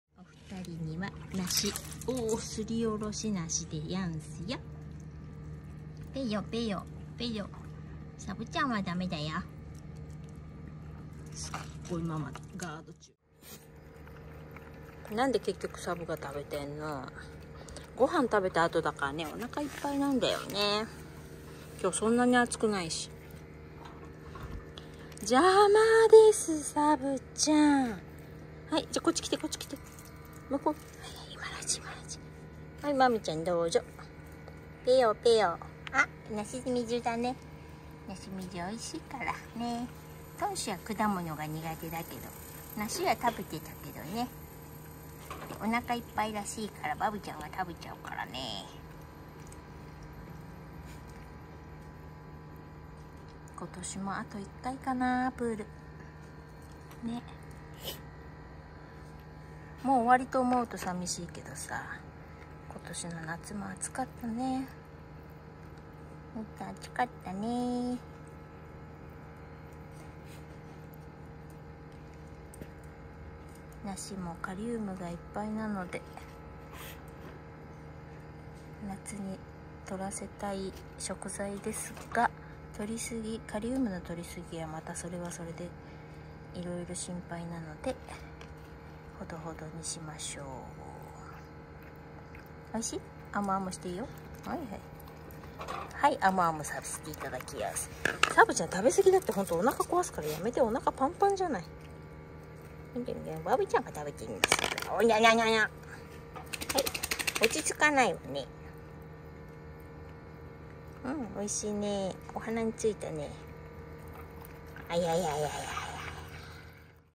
今年３回目のプール④高齢チームはおやつの時間
高齢チームはすりおろし梨にして食べました。